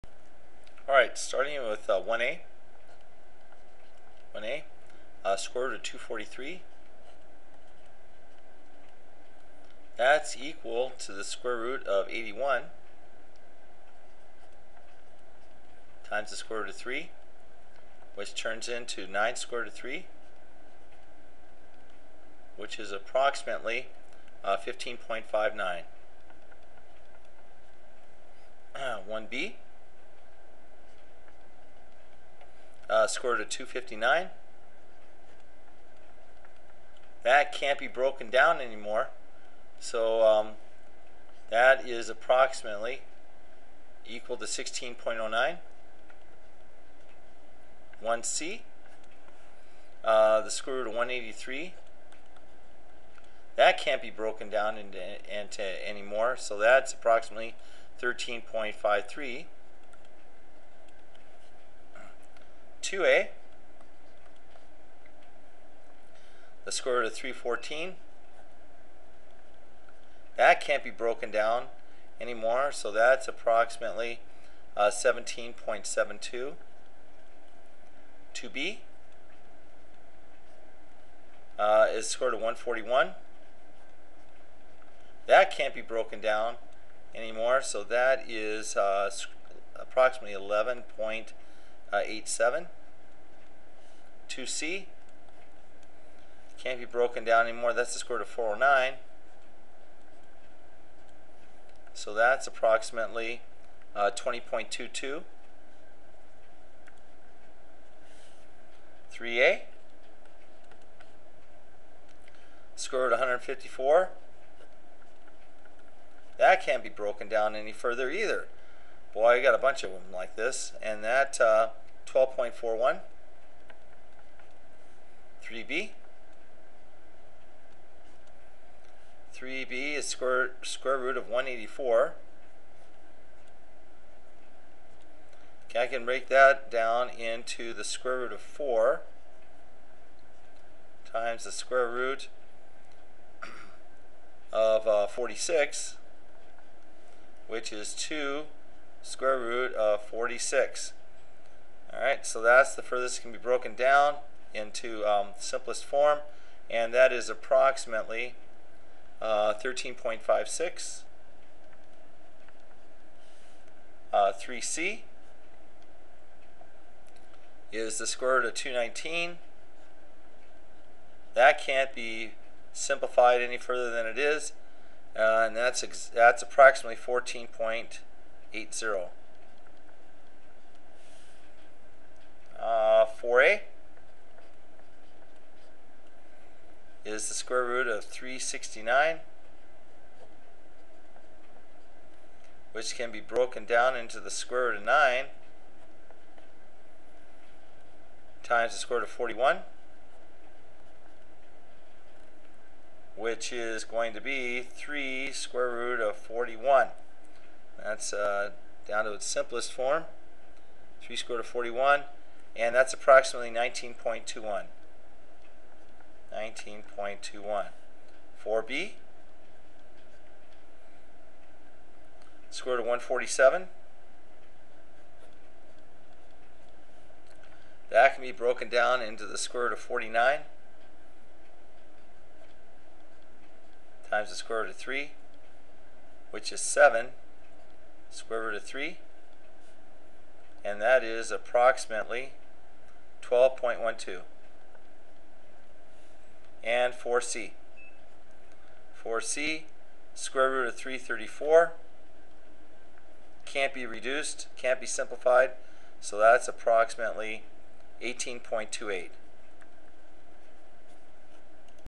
Square Root Worksheet Solutions 1 4
Simplifying Suqare Roots and Finding their decimal approximation